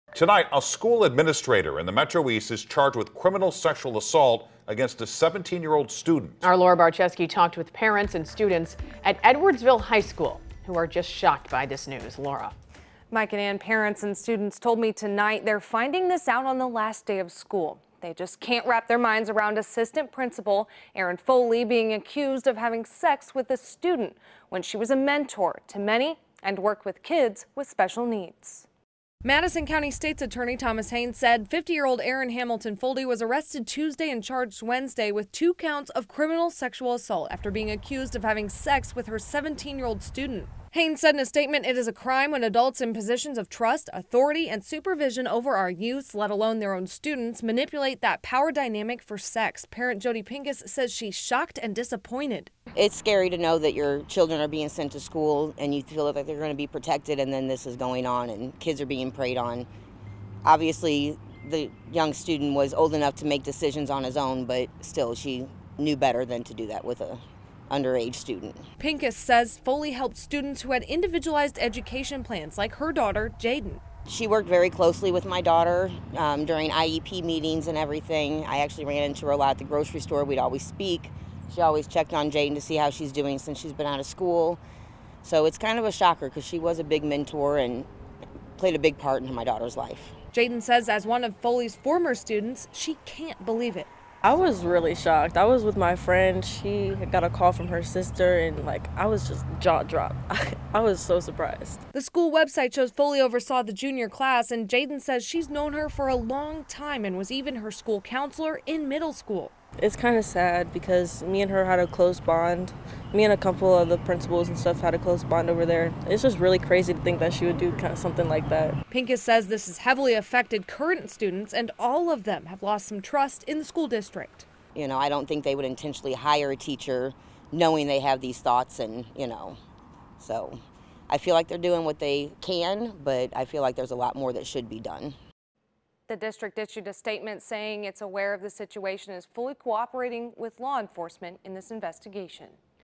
Edwardsville assistant principal accused of sexually assaulting student she was mentoring
A 50-year-old assistant principal was charged with sexual assault after police said she had sexual contact with a student. According to charging documents, Erin Hamilton-Foley was the student's assigned mentor at Edwardsville High School at the time of the alleged sexual assaults.